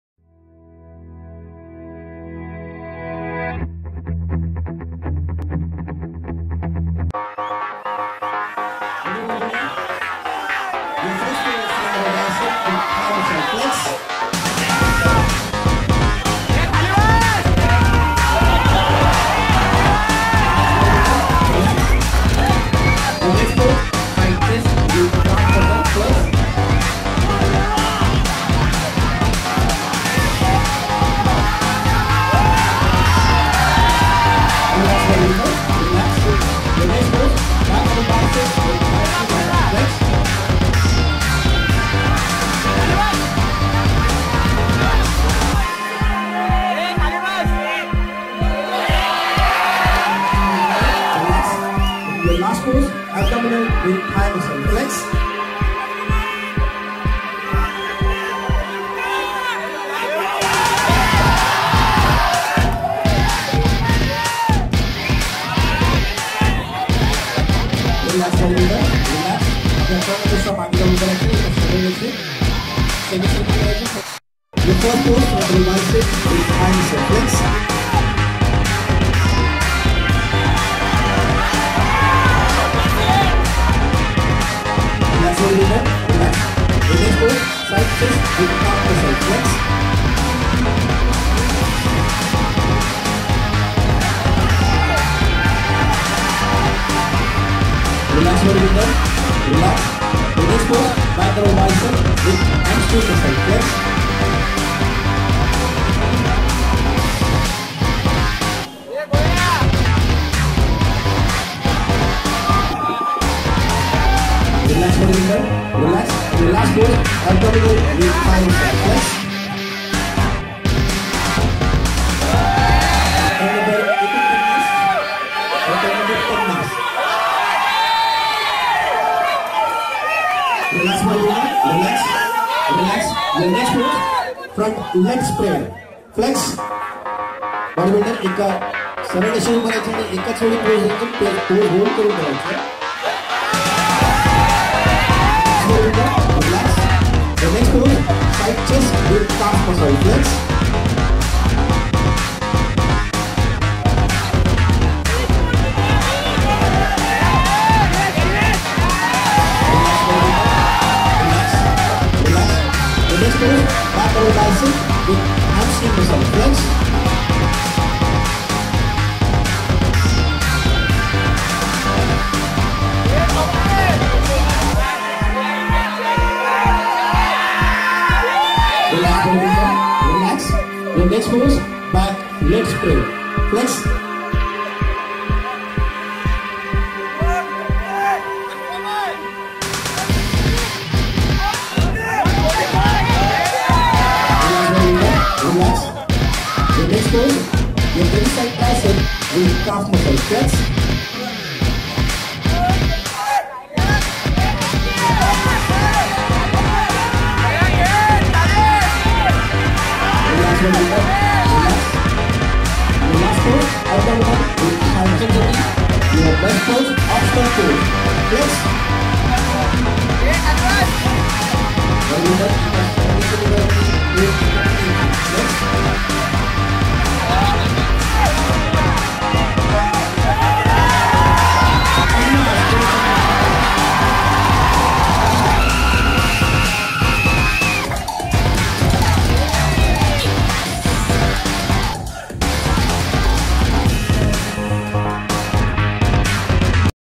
Jogeshwari YuvaShree 2017 (जोगेश्वरी युवाश्री २०१७ ) - Body Building Competition (Part 3 of 7)
Date: 2-April-2017
Venue: Andheri East, Mumbai, Maharashtra, India